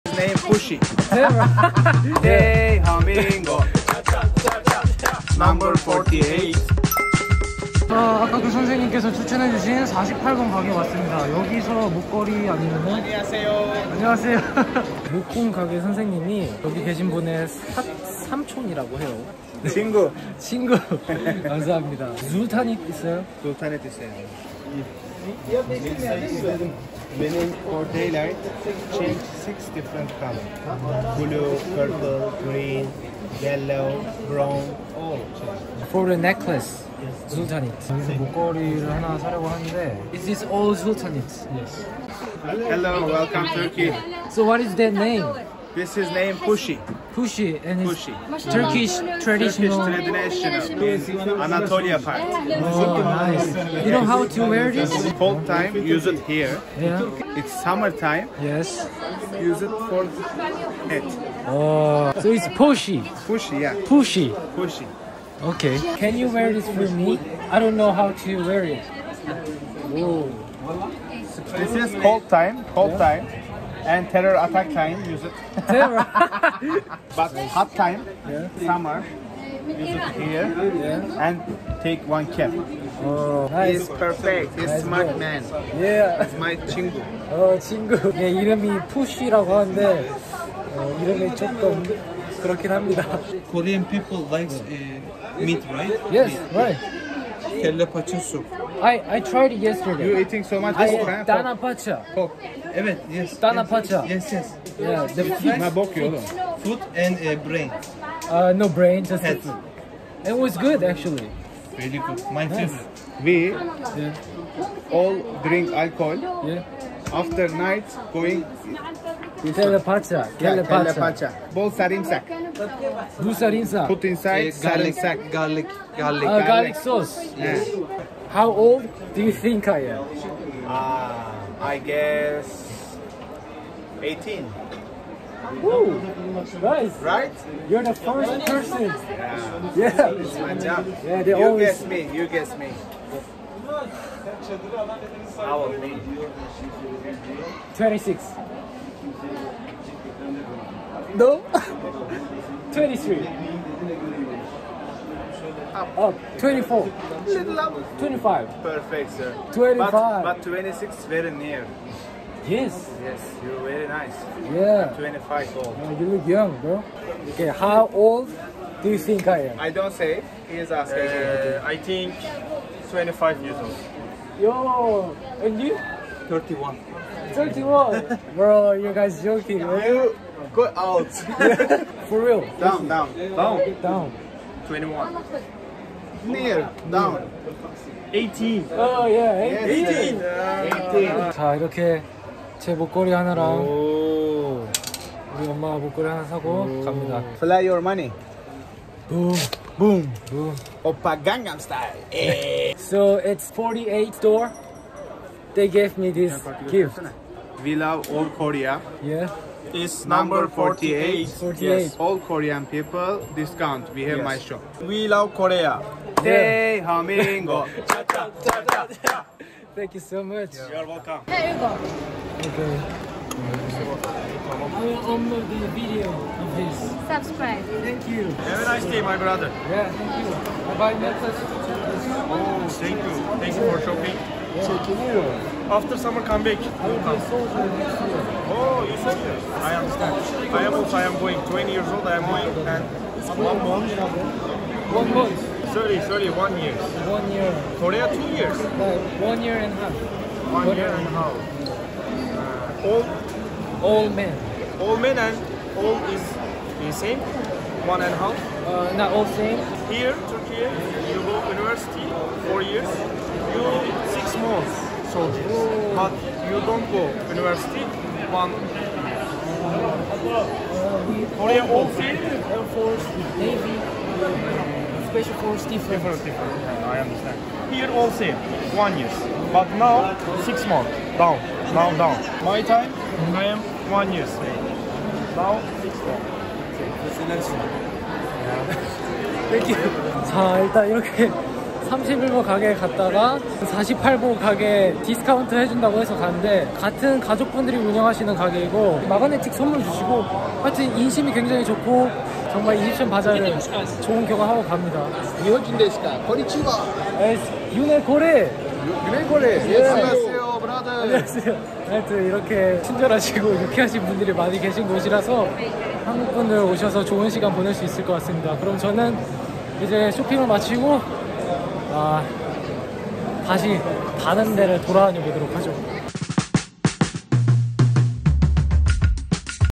미친 텐션의 이스탄불 시장 |🇹🇷튀르키예 VLOG
튀르키예 이스탄불을 여행하며 이집션 바자르 48번 가게에 다녀왔습니다